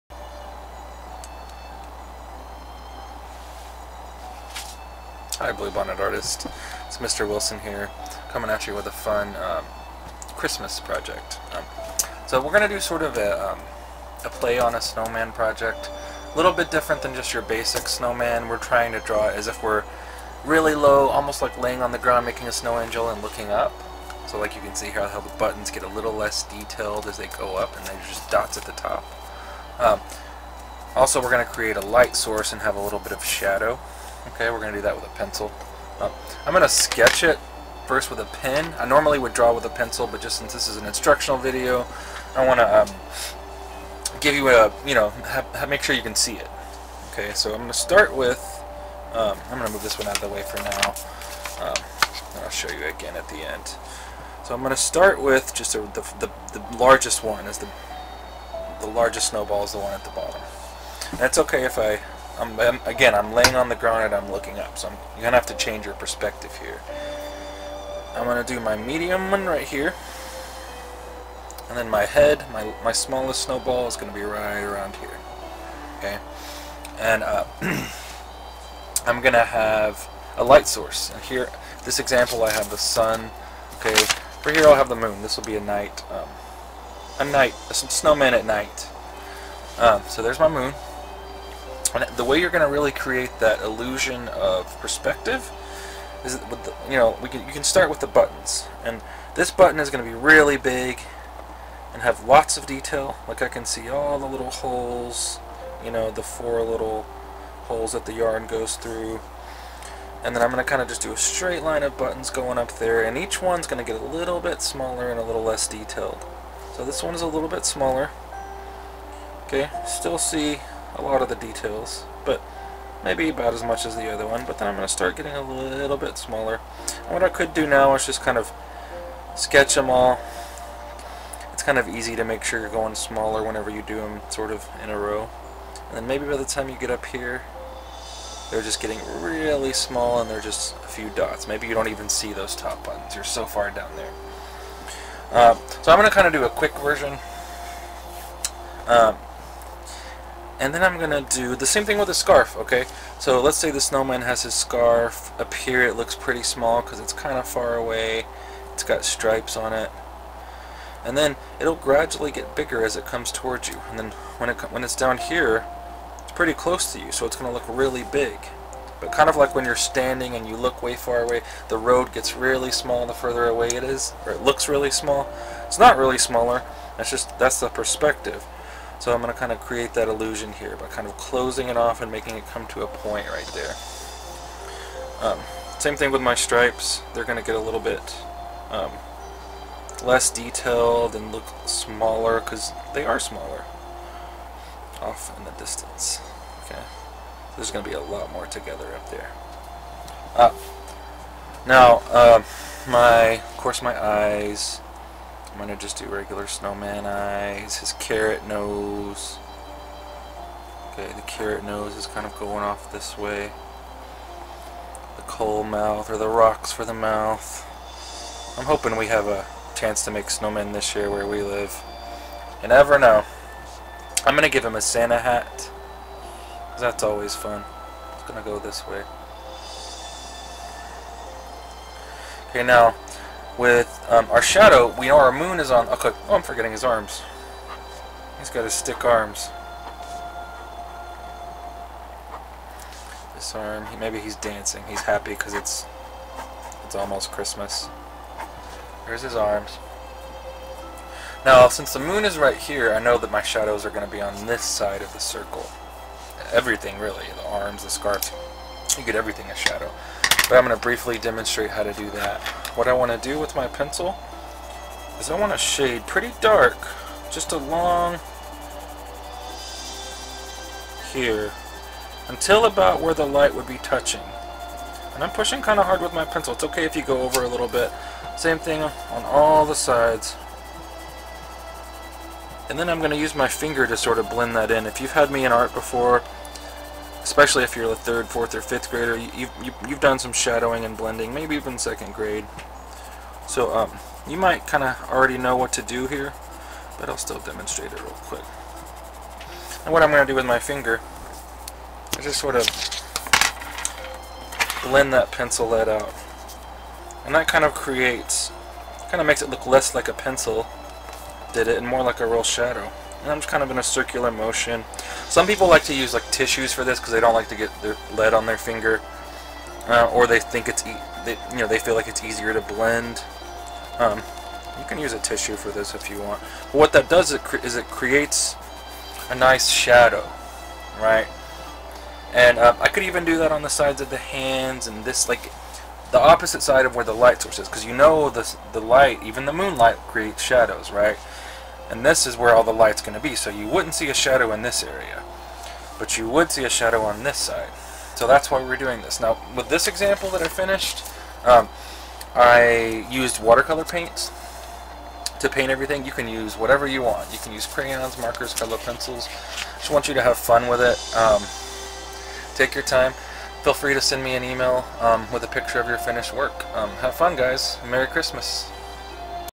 Snowman Demo
Learn how to draw a snowman from a unique perspective while drawing realistic shadows.